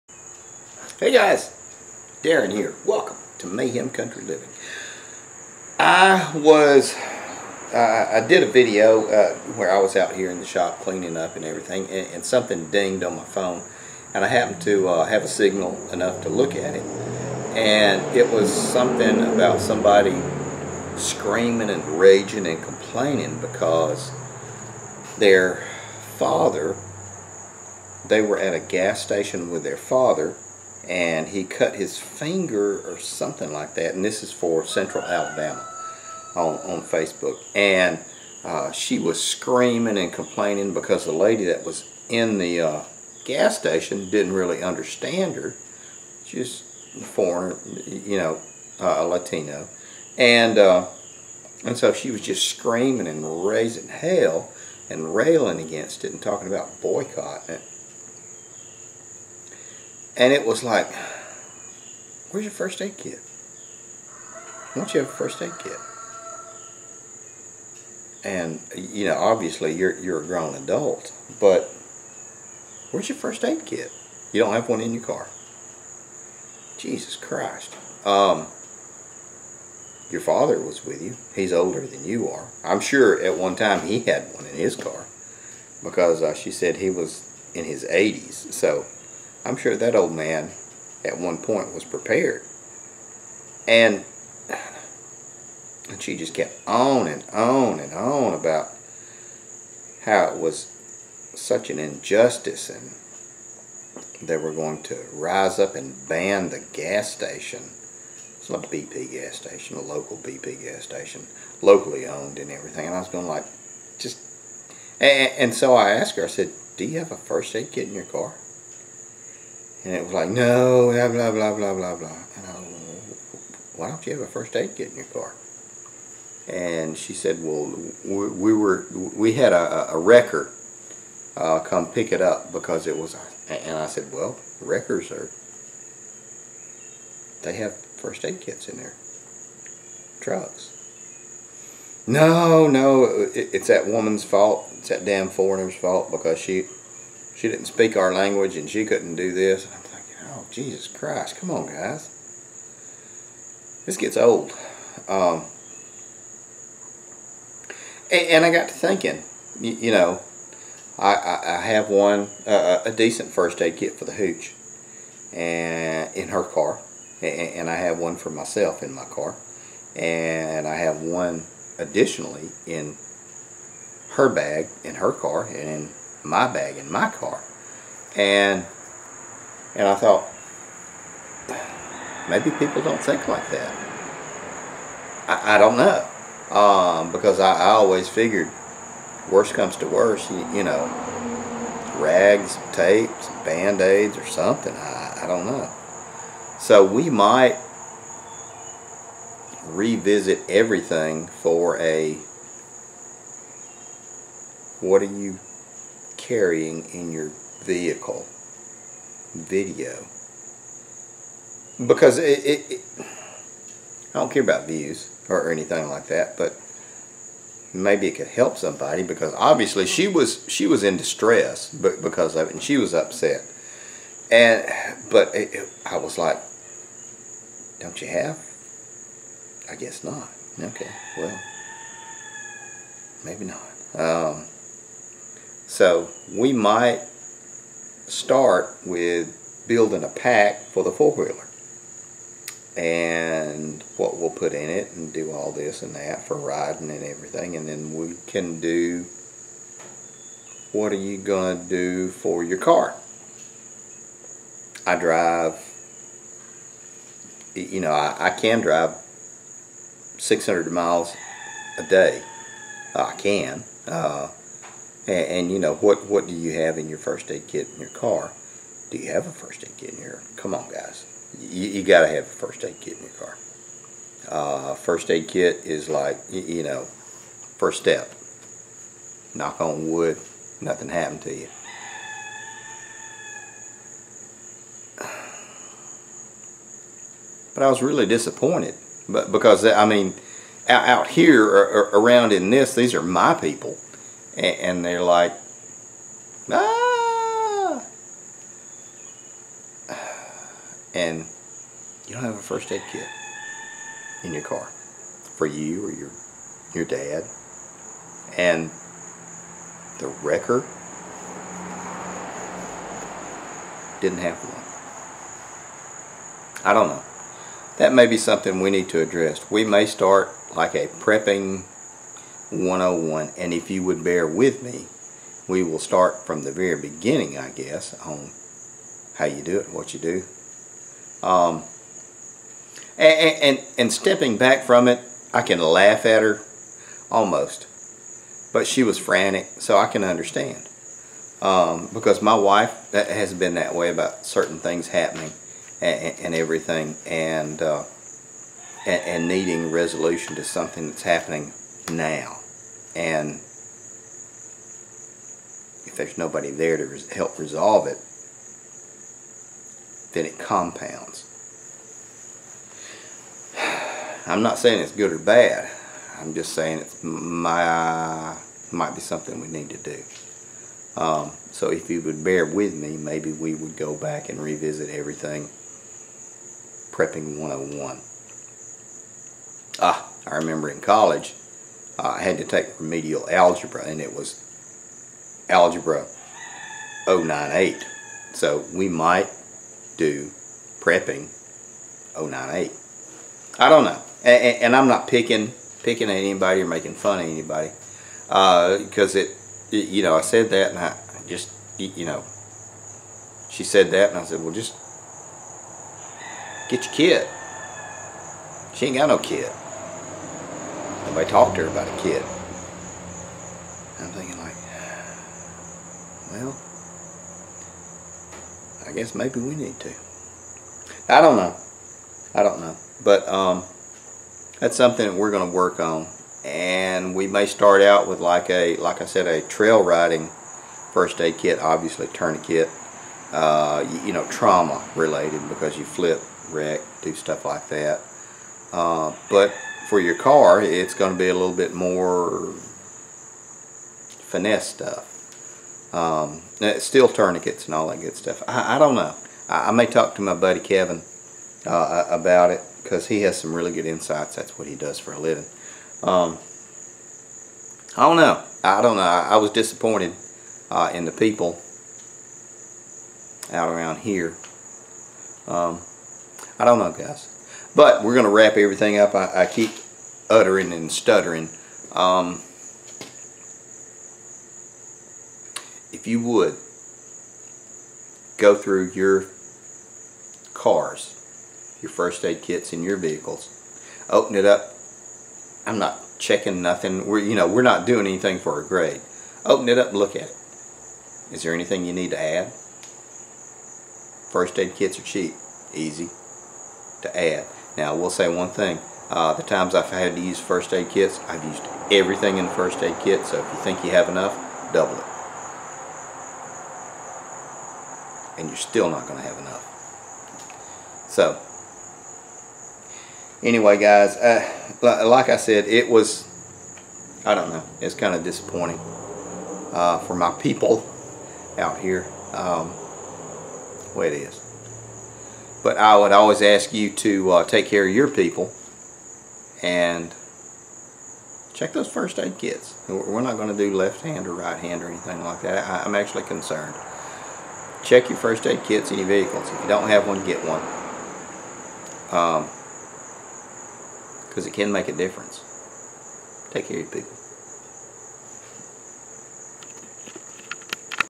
Are you prepared?
Are you prepared? Do you have what you need for an emergency in your car? In your house? We need to have some things covered and not rely on other people for the basics.